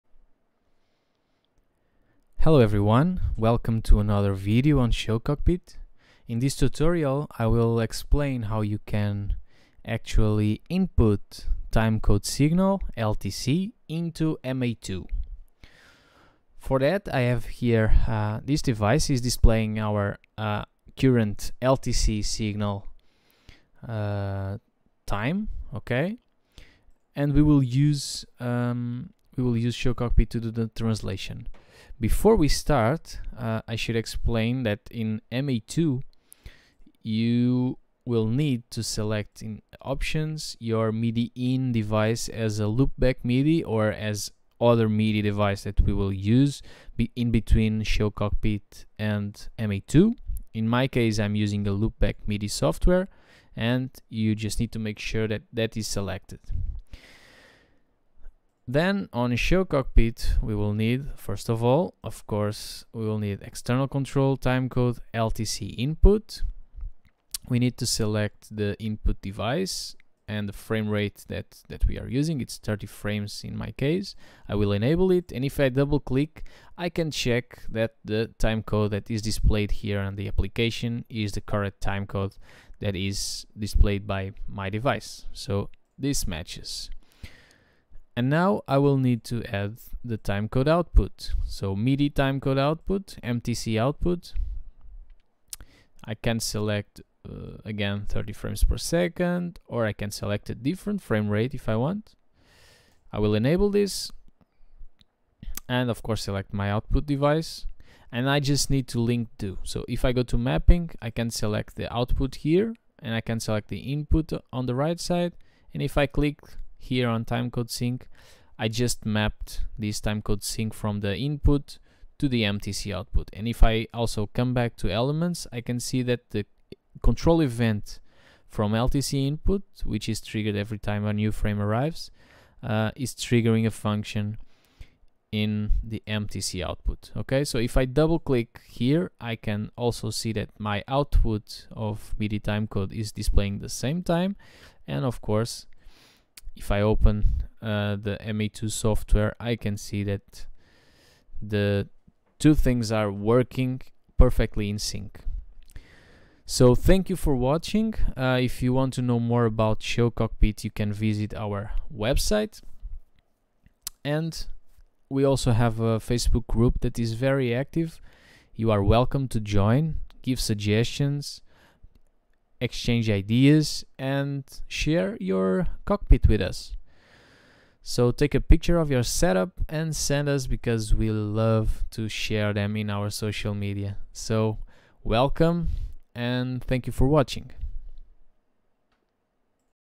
LTC Input to MA2 OnPC
This tutorial explains how to use RD/ShowCockpit to translate LTC Input from the sound card to MTC (MIDI Timecode) and have MA2 OnPC synced with that timecode.